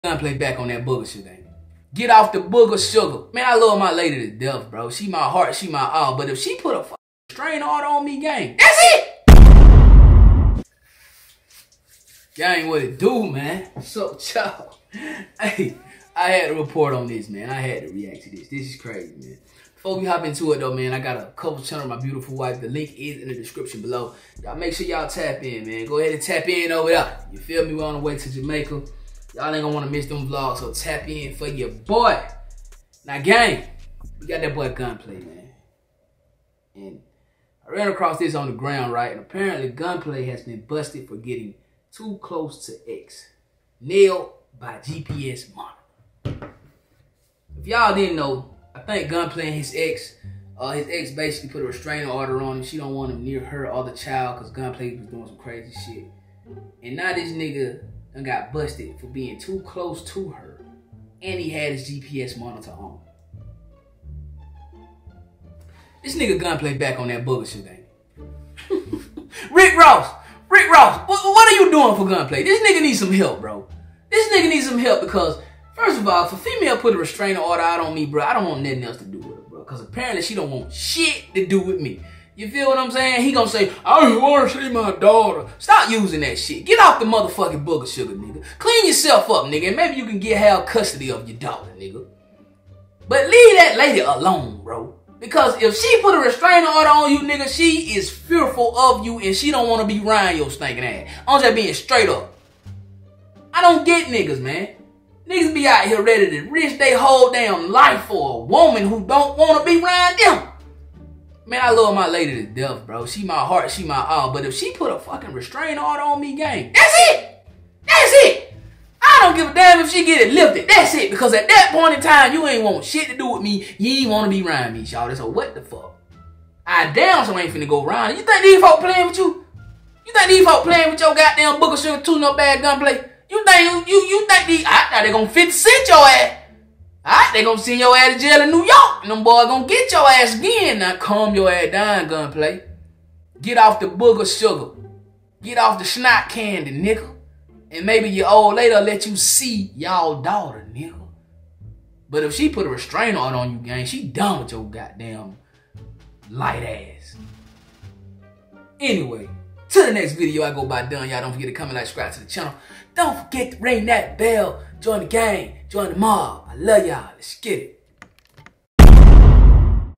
GUNPLAY BACK ON THE NOSE CANDY 👃🏾🍭. BACK ON THE BOOGER SUGER…. OUT HER HARRASSING HIS EX 🤦🏾‍♂️
GANG WHAT UP THOUGH???! TONIGHT WE GOT GUNPLAY… HE OUT HERE ACTING CRAZY GANG STALKING HIS EX 🤦🏾‍♂️

THIS IS MUST SEE TV‼️‼️‼️